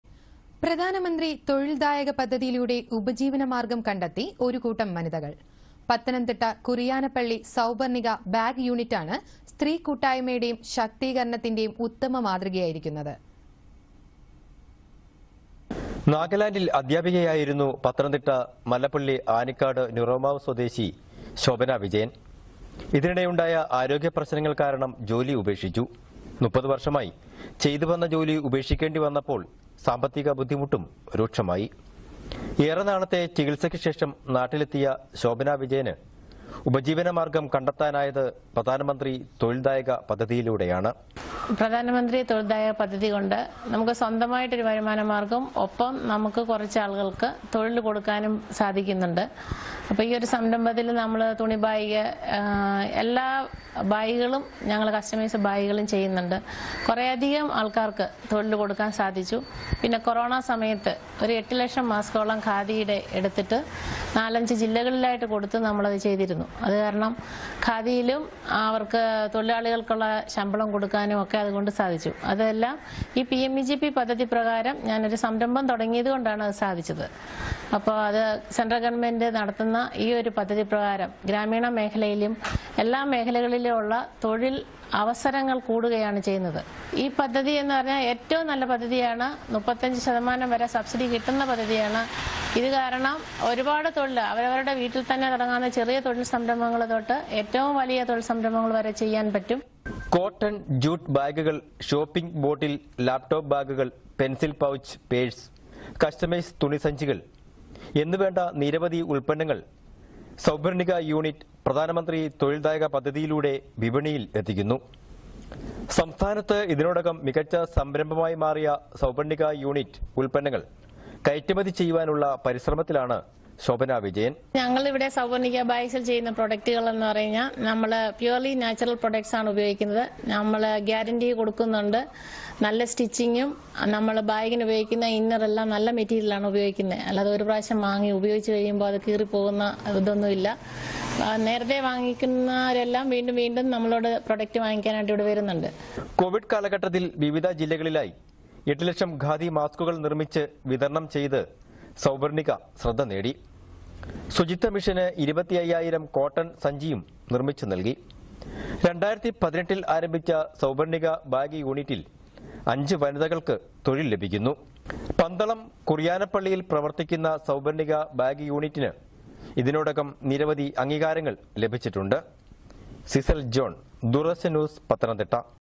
പ്രധാൻ മന്ത്രി തൊഴിൽ ദായക പദ്ധതിയിലൂടെ ഉപജീവന മാർഗ്ഗം കണ്ടെത്തി ഒരു കൂട്ടം വനിതകൾ
പത്തനംതിട്ട കുറിയാനപ്പള്ളി സൗപർണ്ണിക ബാഗ് യൂണിറ്റാണ് സ്ത്രീ കൂട്ടായ്മയുടെയും ശാക്തീകരണത്തിന്റെയും ഉത്തമ മാതൃകയായിരിക്കുന്നത്.